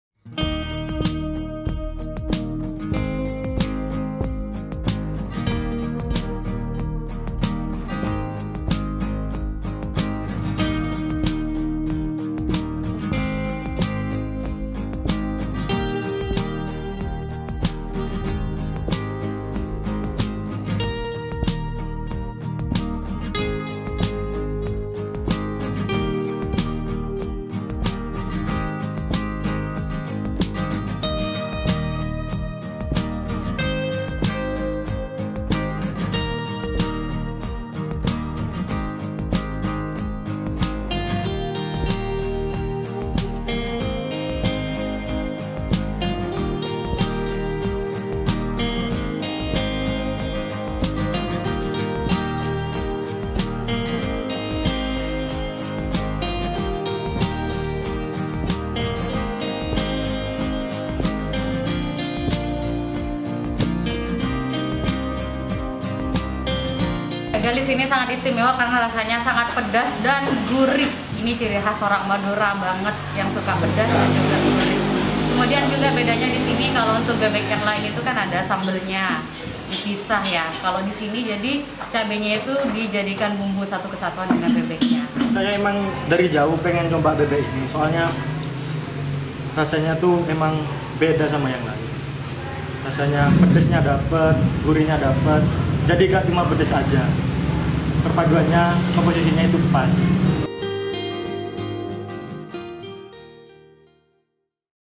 Lezatnya Bebek Pedas Galis Khas Bangkalan
Bangkalan, KompasTV Jawa Timur - Anda penggemar kuliner pedas? di warung Bebek Pedas Galis Kabupaten Bangkalan, Jawa Timur tempatnya. Dengan uang delapan belas ribu rupiah, satu piring nasi dan Bebek Pedas Galis sudah bisa dinikmati.

Masa pandemi covid 19 yang berkepanjangan banyak tempat usaha kuliner tutup karena tidak ada pembeli yang masuk ke warung untuk makan.

Namun kini bisnis kuliner mulai menggeliat, pemilik warung makan mulai berlomba menarik minat pembeli, dengan menyajikan menu khusus seperti di warung Bebek Pedas Galis. Warung yang dikelola Anna dan keluarga, yang berada di Kota Bangkalan ini, sengaja menyajikan masakan yang serba pedas, seperti Bebek Pedas Galis.

Bebek yang sudah digoreng hingga kering ini, kemudian dicampur bumbu pedas sehingga sangat nikmat saat disantap bersama nasi putih maupun nasi jagung. Keringat langsung bercucuran saat menyantap bebek yang sudah dicampur bumbu pedas dengan rempah khusus khas warung bebek galis ini.

Untuk mendapatkan satu piring nasi dengan satu potong bebek pedas, cukup sediakan uang delapan belas ribu rupiah saja. Tak hanya dimakan di tempat, pengunjung bebek pedas ini juga banyak yang pesan secara online.